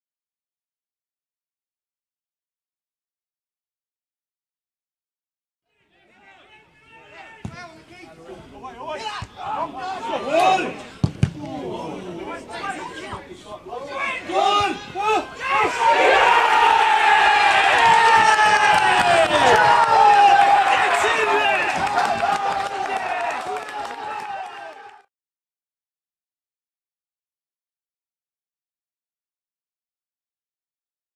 Rowan Liburd scores against Margate
Billericay Town 4 Margate 3
Ryman League, Premier Division.  Tuesday 14 April 2015.